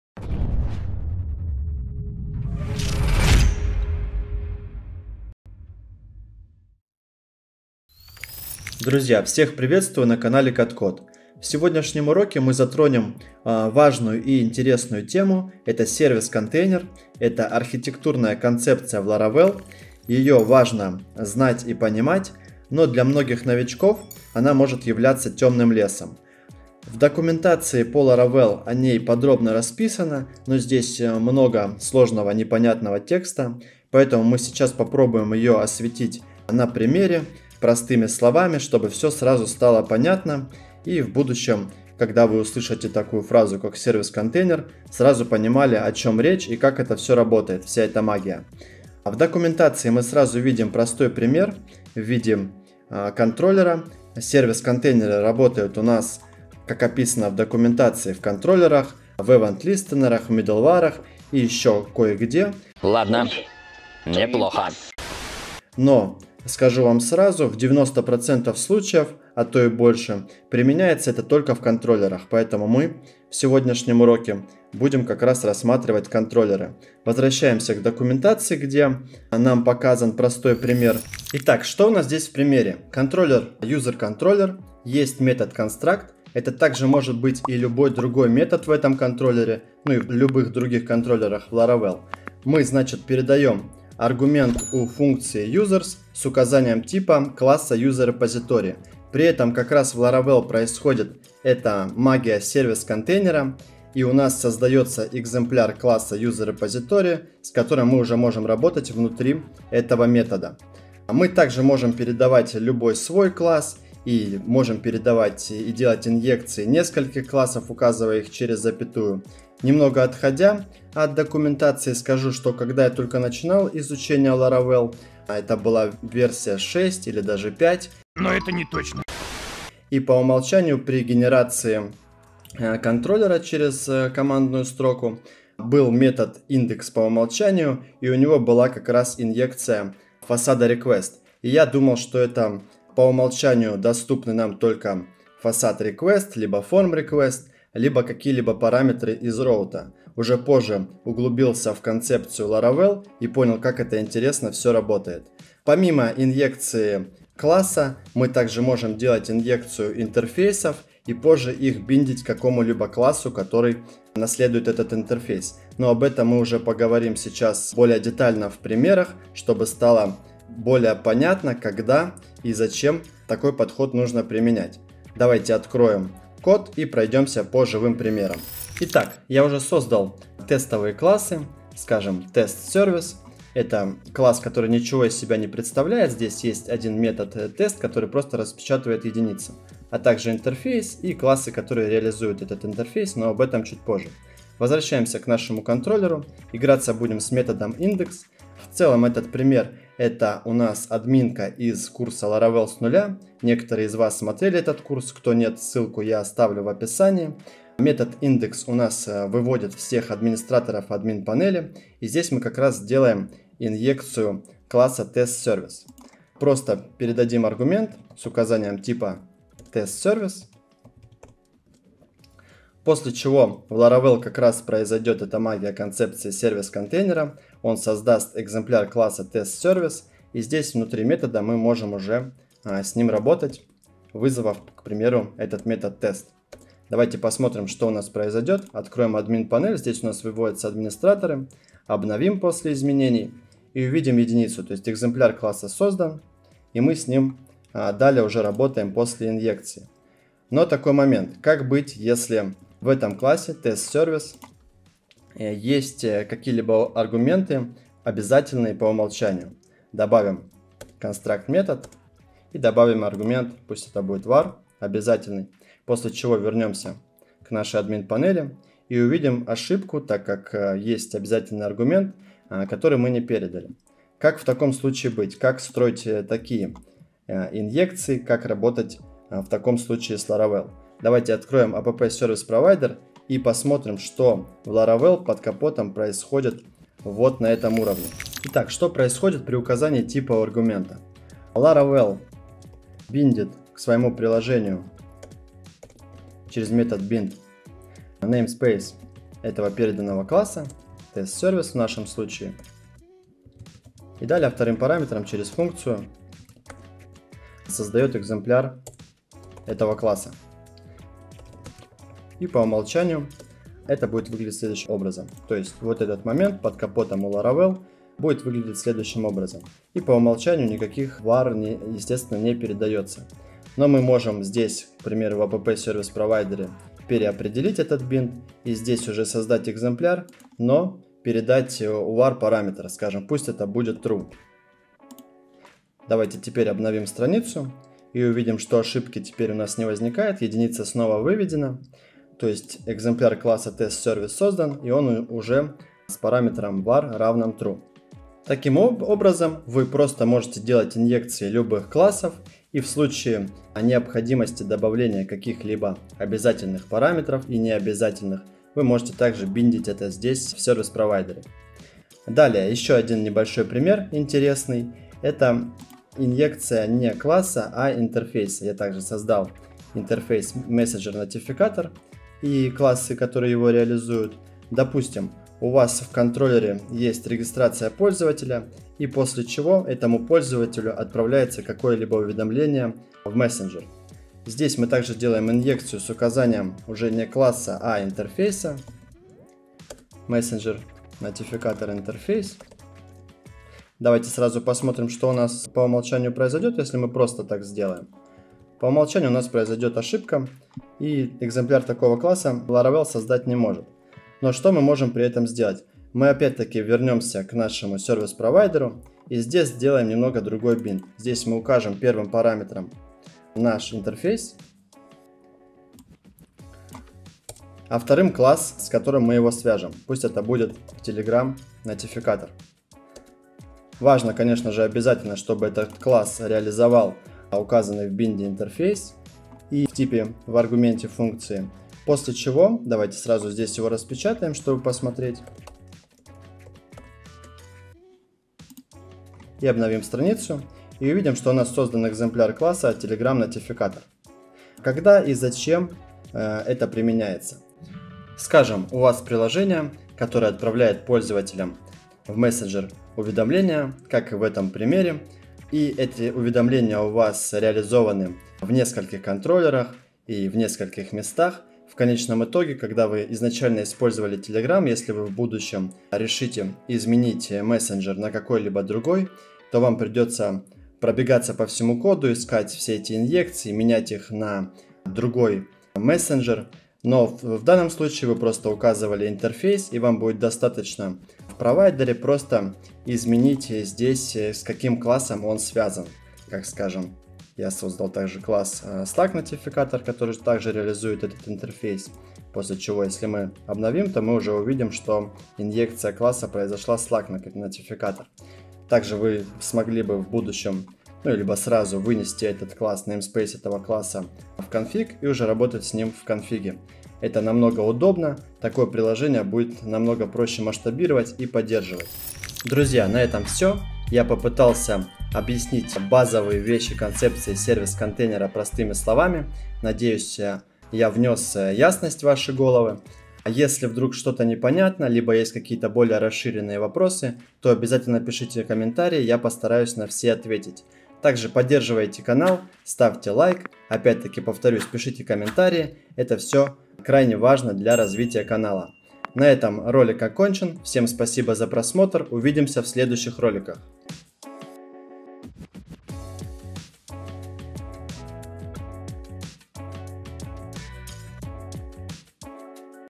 Service container в Laravel - архитектурная концепция и инъекции зависимостей в Ларавел
В сегодняшнем уроке мы затронем важную и интересную тему это service container (ларавел сервис контейнер). Это архитектурная концепция в Laravel, её важно знать и понимать, но для многих новичков она может являться темным лесом.

❗️❗️❗️как делать сложные сайты на laravel? Это просто с cutcode!

🤖🤖🤖Мой помощник Тэйлор готов выдать тебе подарок.

⏰ Таймкоды:
00:00 Введение
00:27 Обзор документации по Laravel
02:00 Небольшая история как я изучал service container много лет назад
02:58 Пример с инъекцией класса
04:48 Что происходит при указании типа аргумента
06:01 Пример с инъекцией интерфейса

Друзья всех приветствую на канале Cutcode! В сегодняшнем уроке мы затронем важную и интересную тему это service container (ларавел сервис контейнер). Это архитектурная концепция в Laravel, её важно знать и понимать, но для многих новичков она может являться темным лесом.

В документации по Laravel о ней подробно расписано, но здесь много сложного непонятного текста. Поэтому мы сейчас попробуем ее осветить на примере простыми словами, чтобы все сразу стало понятным и в будущем когда вы услышите такую фразу как сервис контейнер сразу понимали о чем речь и как это все работает, вся эта магия.

В документации мы сразу видим простой пример видим контроллера service container работают у нас как описано в документации в контроллерах, в event листнерах, мидлварах и еще кое-где. Но скажу вам сразу в 90 процентов случаев, а то и больше применяется это только в контроллерах. Поэтому мы сегодняшнем уроке будем как раз рассматривать контроллеры.

Возвращаемся к документации где а нам показан простой пример. В примере контроллере user есть метод construct. Это так же может быть и любой другой метод в этом контроллере. Ну и в любых других контроллерах Laravel.

Мы значит передаем аргумент функции users с указанием типом класс users репозитории. При этом как раз в Laravel происходит это магия service container и у нас создается экземпляр класса user репозитории с которой мы уже можем работать внутри этого метода. Мы также можем передавать любой свой класс и можем передавать и делать инъекции нескольких классов, указывая их через запятую. Немного отходя от документации скажу что когда я только начинал изучение Laravel а это была версия 6 или даже 5 и по умолчанию при генерации контроллера через командную строку был метод index по умолчанию и у него было как раз инъекция фасада request. И я думал что это по умолчанию доступны нам только фасад request, либо форм request либо какие-либо параметры из route, а уже позже углубился в концепцию Laravel и понял как это интересно все работает.

Помимо инъекции класса мы также можем делать инъекцию интерфейсов и позже их биндить какому-либо классу который наследует этот интерфейс. Но об этом мы уже поговорим сейчас более детально в примерах, чтобы стало более понятно, когда и зачем такой подход нужно применять.

Давайте откроем код и пройдемся по живым примерам.

Service conteiner в Laravel - архитектурная концепция и инъекции в Ларавел